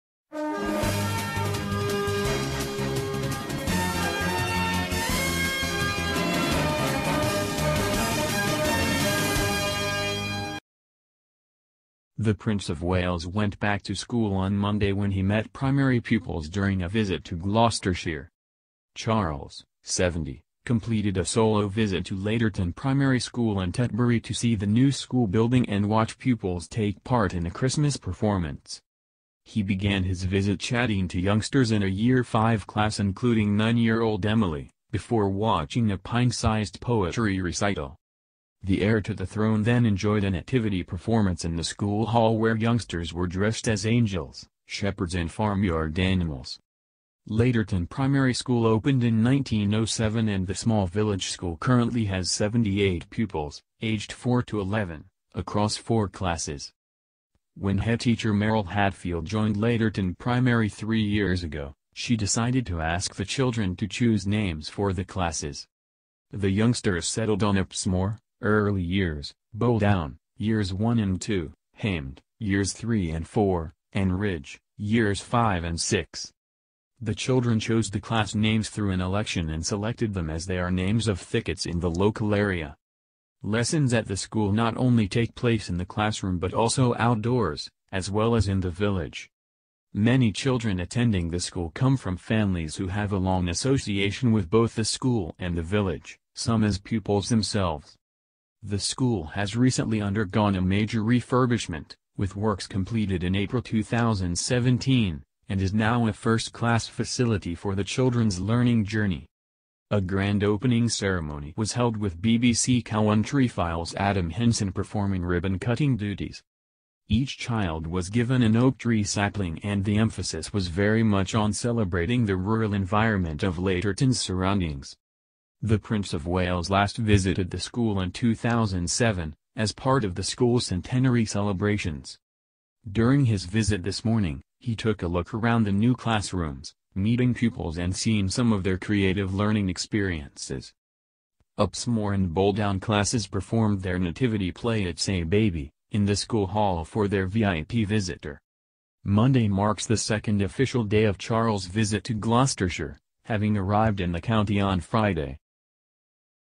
Back to school! Prince of Wales looks friendly when he meets primary pupils in Gloucestershire
Back to school! Prince of Wales meets primary pupils in Gloucestershire where he watches a pint-sized poetry recital and a sweet nativity play